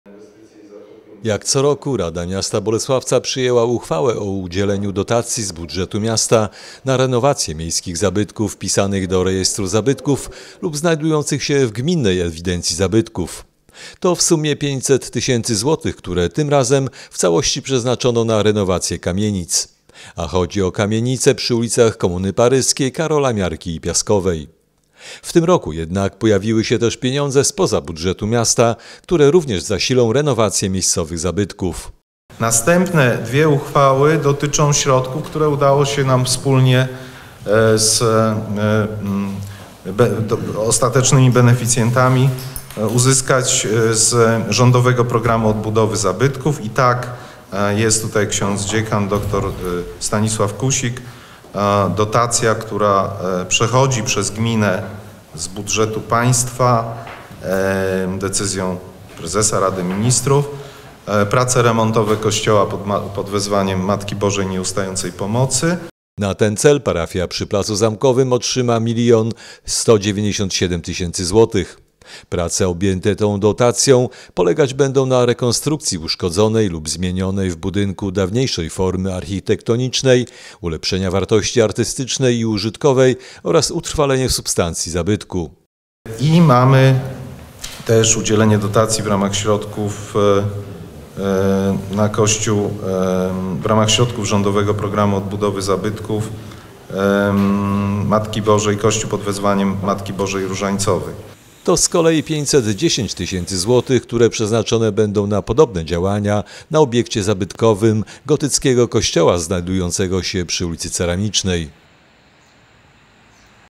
Dotacje na zabytki
Radni miejscy rozdzielili dotacje na renowację zabytków w Bolesławcu. 500 tys. złotych z miejskiego budżetu zasili remonty kamienic. Ale pojawiły się też rządowe pieniądze, które wspomogą renowację dwóch kościołów: przy placu Zamkowym i przy ulicy Ceramicznej. To w sumie dodatkowo ponad 1,7 mln złotych.